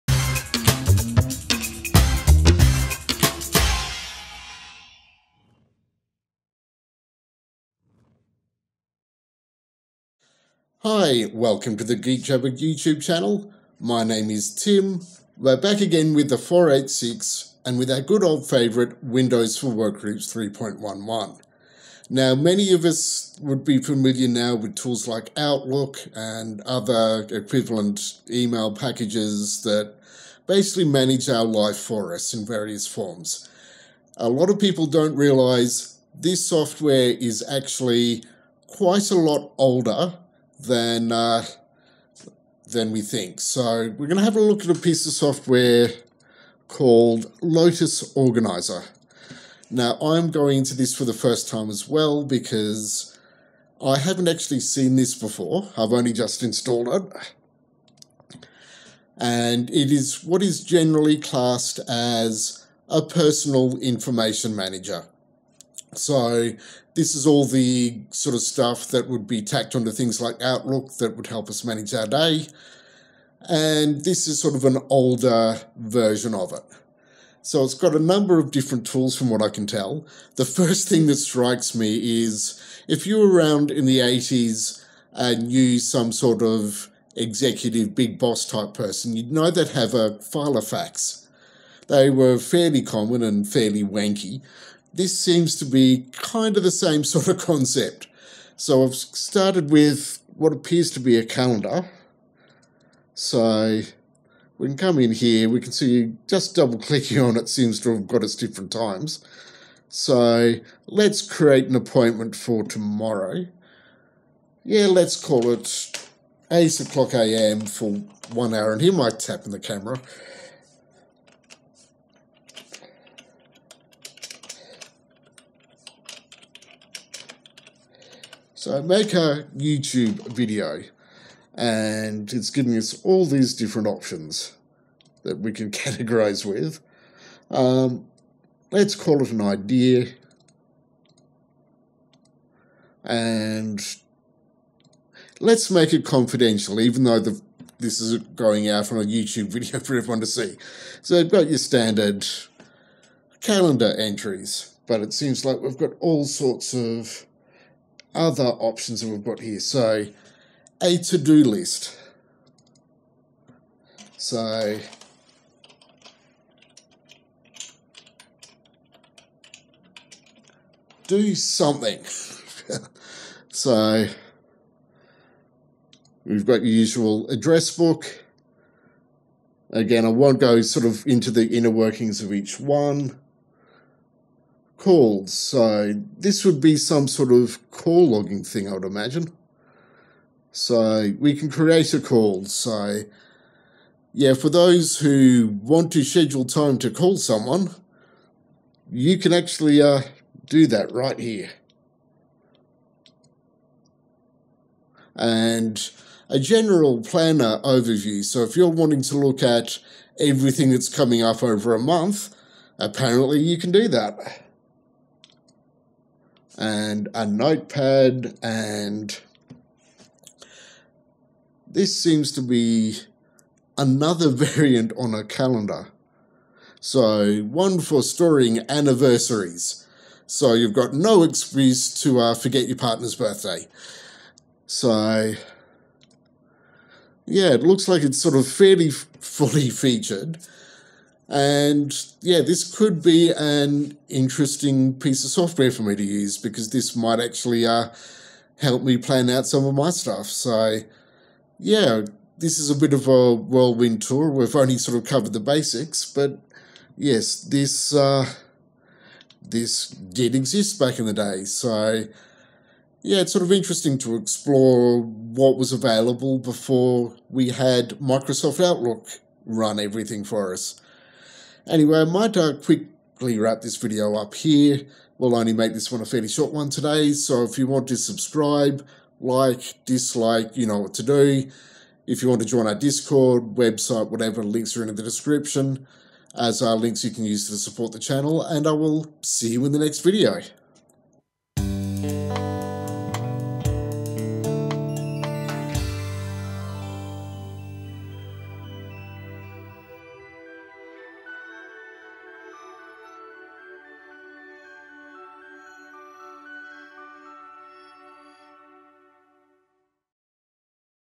Lotus Organiser for Windows
In this episode, we look at Lotus Organiser, an early Personal Information Manager (or PIM) for Windows.